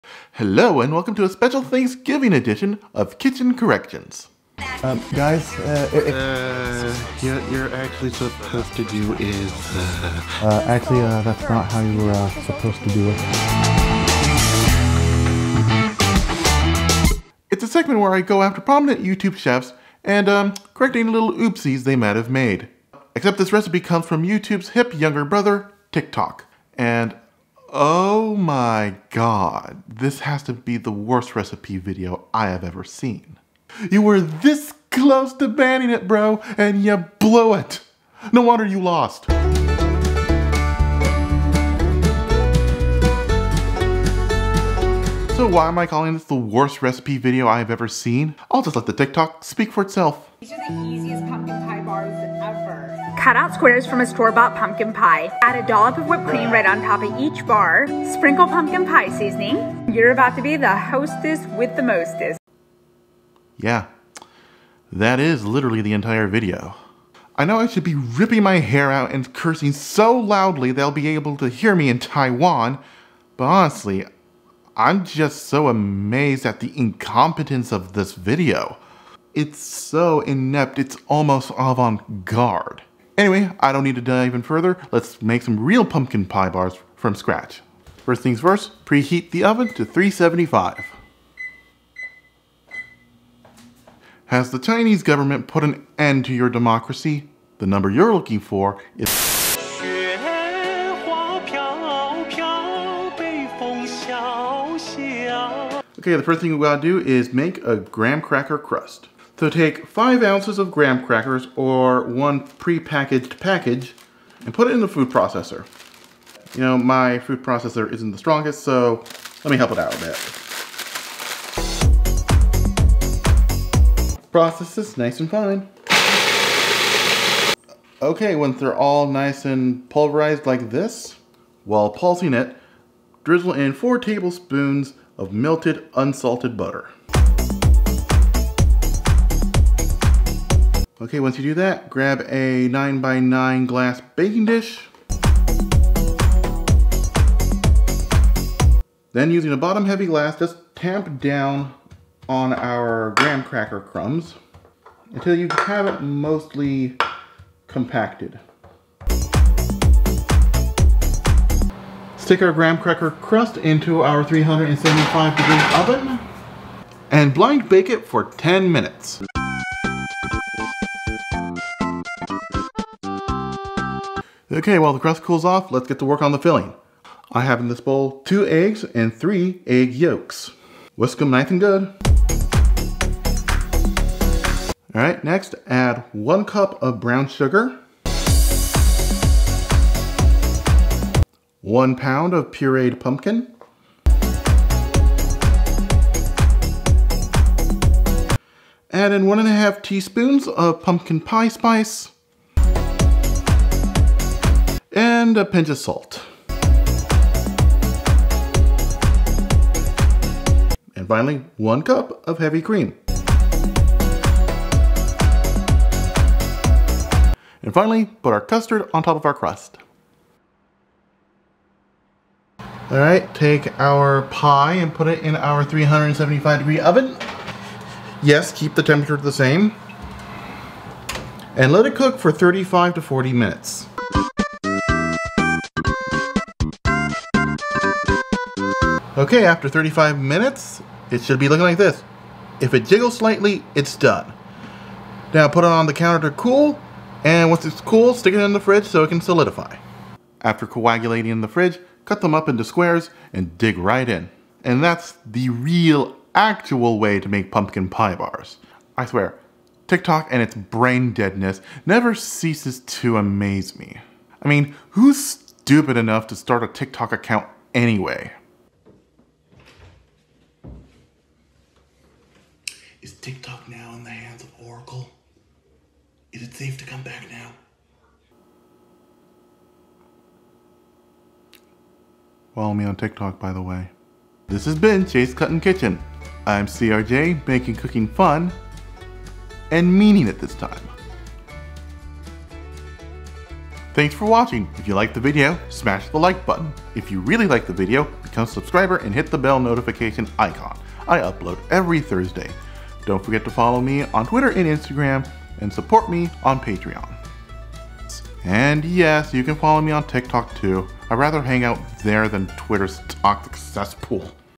Pumpkin Pie Bars Kitchen Korrections
The other day I was browsing TikTok, and came accross what has to be the worst recipe video I have ever seen.  Basically, a "chef" was "making" pumpkin pie bars and completely botched it.  Since pumpkin pie in bar form is a wonderful idea, I took it upon myself to make a proper recipe video.